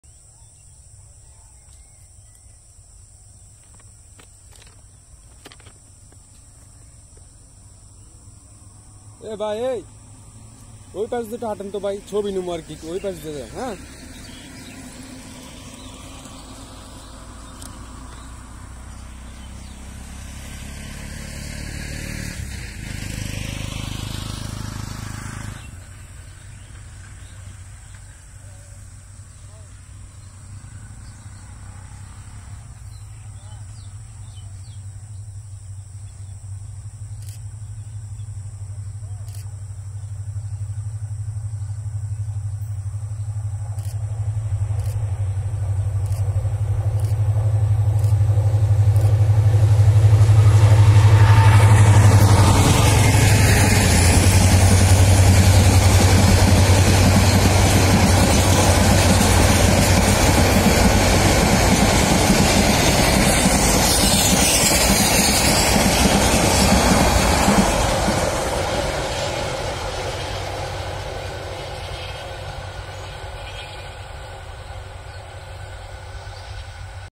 পারাবত এক্সপ্রেস ভুলেট গতিতে মেথিকান্দা প্রবেশ করে (parabat express)
parabat express through winter mathikanda Railway station